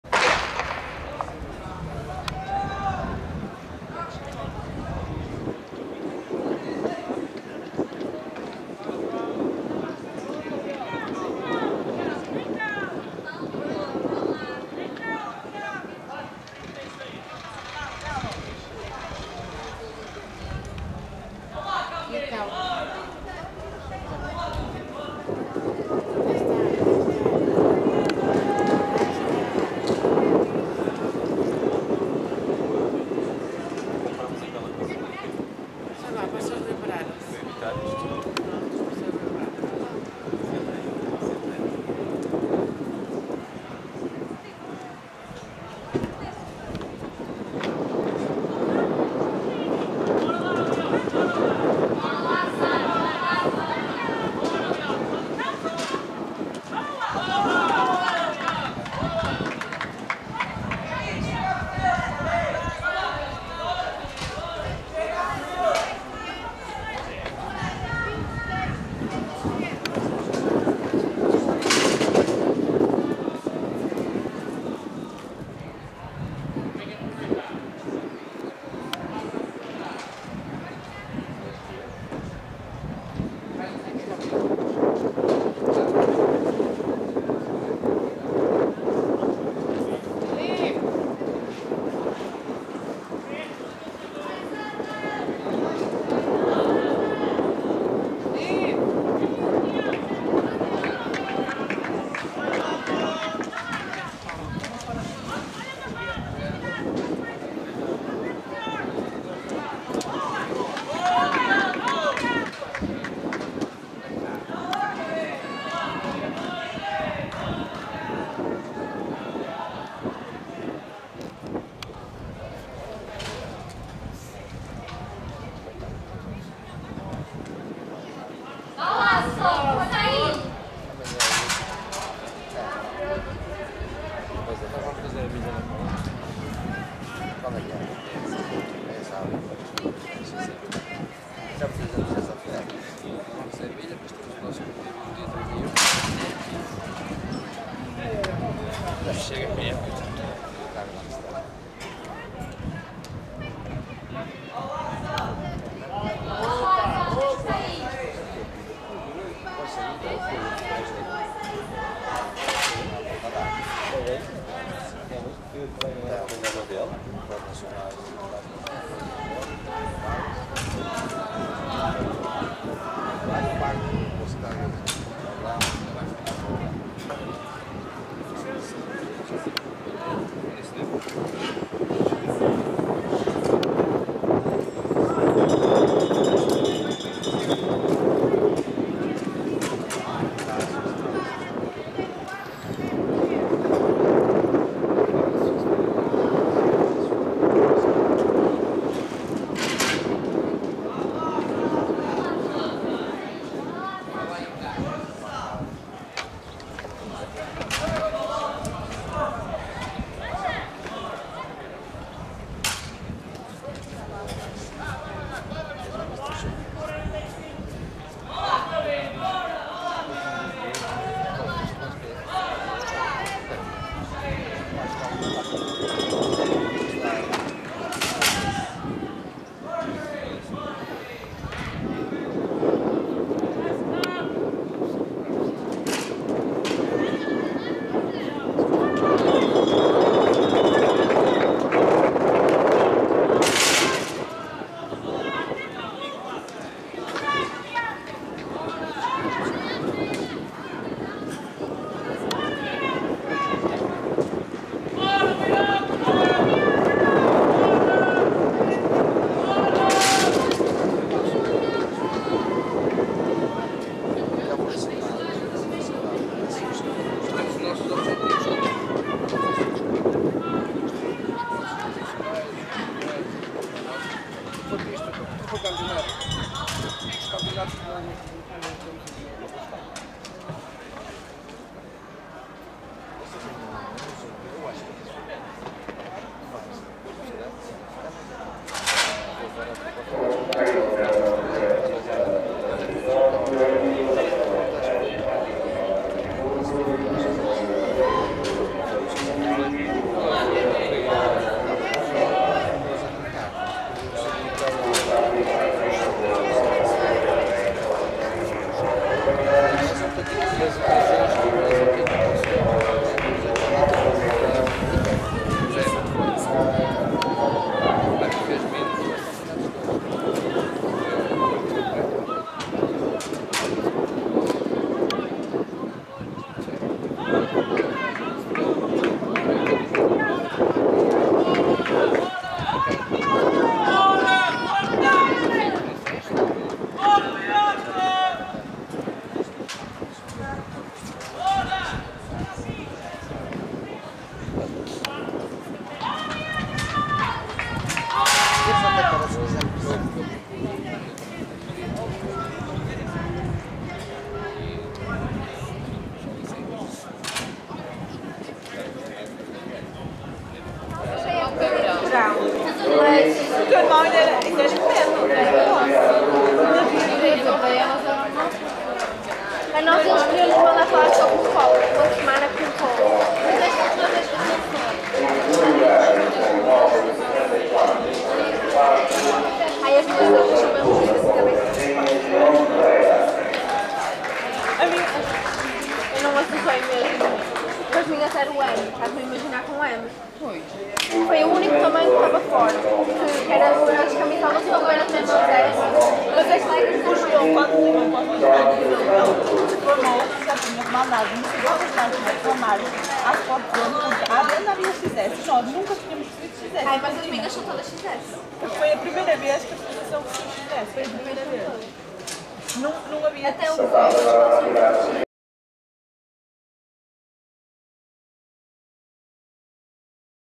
Atletismo - Camp. Norte 2016: 3000 metros Obstáculos masculinos
Atletismo - Campeonato Absoluto do Norte 2016: 3000 metros Obstáculos masculinos. Prova disputada na Pista da Maia, na manhã do dia 24 de Julho de 2016.